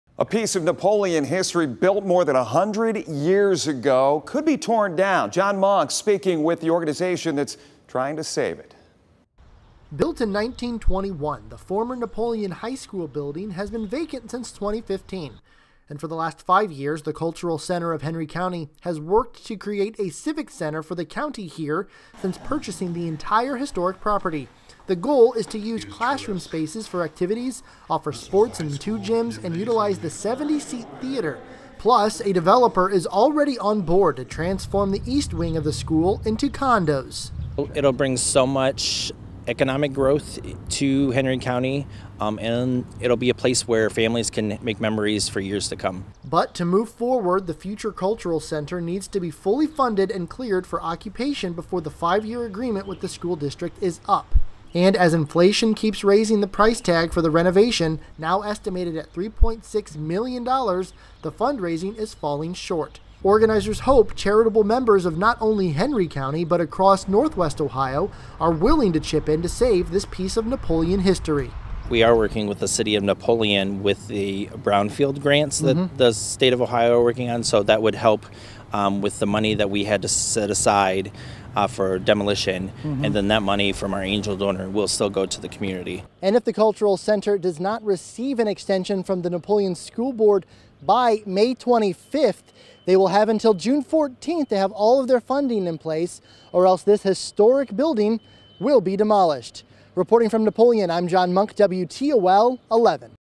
Plans for Henry County civic center falling short on fundraising
A piece of Napoleon history built more than 100 years ago could be torn down.

Built in 1921, the former Napoleon High School building has been vacant since 2015.

And for the last five years, the Cultural Center of Henry County has worked to create a civic center for the county here since purchasing the entire historic property.

But to move forward, the future cultural center needs to be fully funded and cleared for occupation before the five-year agreement with the school district is up.